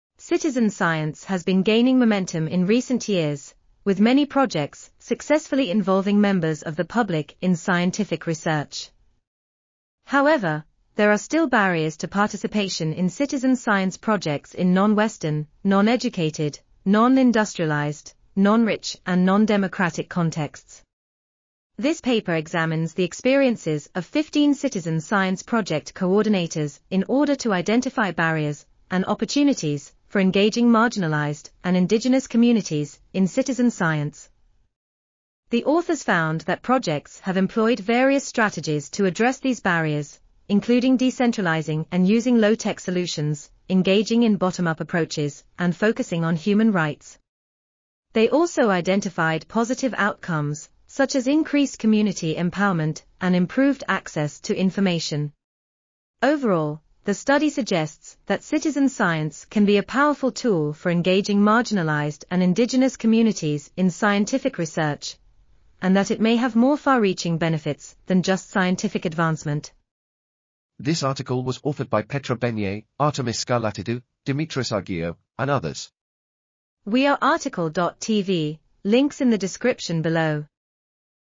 Challenges, Strategies, and Impacts of Doing Citizen Science with Marginalised and In... | RTCL.TV
### Keywords ###

### Article Attribution ###
Title: Challenges, Strategies, and Impacts of Doing Citizen Science with Marginalised and Indigenous Communities: Reflections from Project Coordinators
Authors: Petra Benyei, Artemis Skarlatidou, Dimitris Argyriou, Rick Hall, Ida Theilade, Nerea Turreira-García, Danielle Latreche, Alexandra Albert, David Berger, Mar Cartró-Sabaté, Jessie Chang, Rafael Chiaravalloti, Arianna Cortesi, Finn Danielsen, Muki (Mordechai) Haklay, Emily Jacobi, Asaye Nigussie, Victoria Reyes-García, Eliana Rodrigues, Thamara Sauini, Vyacheslav Shadrin, Andre Siqueira, Mr. Supriadi, Mardha Tillah, Adrien Tofighi-Niaki, Nikita Vronski ,and Timothy Woods
Publisher: Ubiquity Press
DOI: 10.5334/cstp.514

### Video Timestamps ###
0:00:00 - Summary
0:01:10 - Title
0:01:16 - Outro
0:01:21 - End